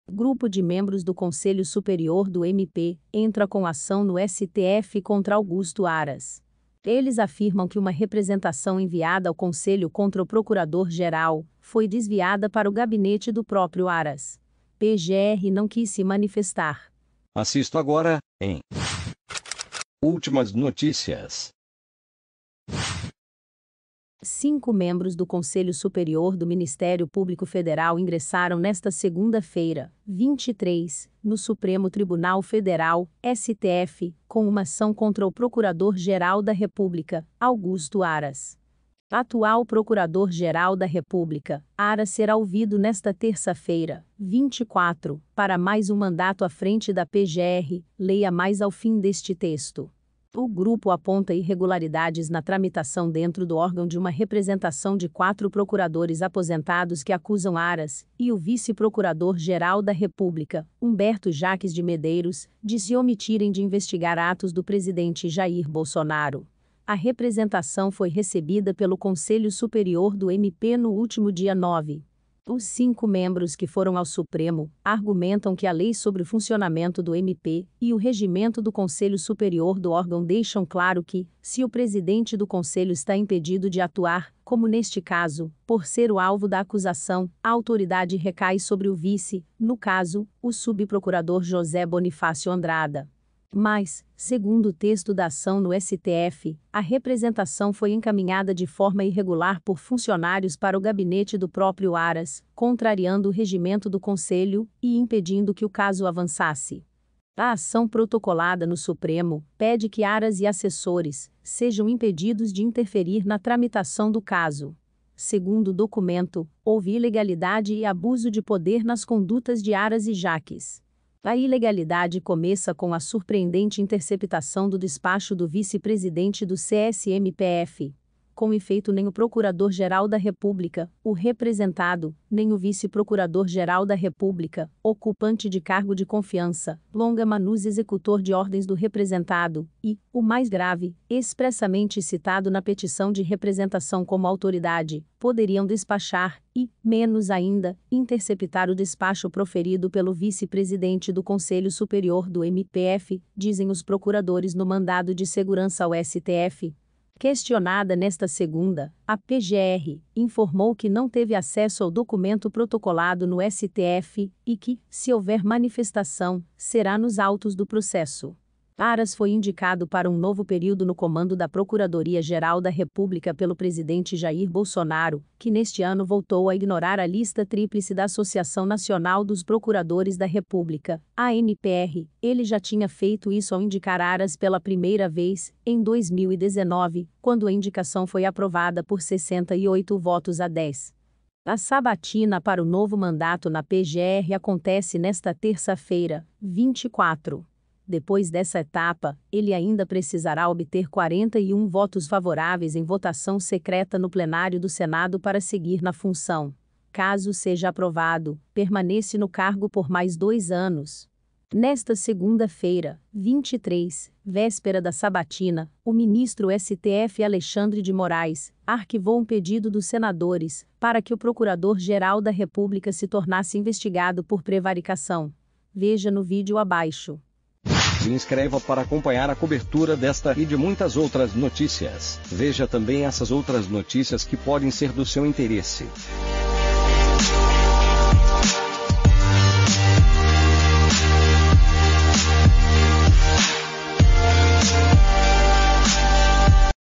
Grupo de membros do Conselho Superior do MP entra com ação no STF contra Augusto Aras
Eles afirmam que uma representação enviada ao conselho contra o procurador-geral foi desviada para o gabinete do próprio Aras. PGR não quis se manifestar.

🔴 Quer ELIMINAR entre 5 e 10 KG em 30 dias?

Acessado em: 24 de agosto de 2021 às 08:34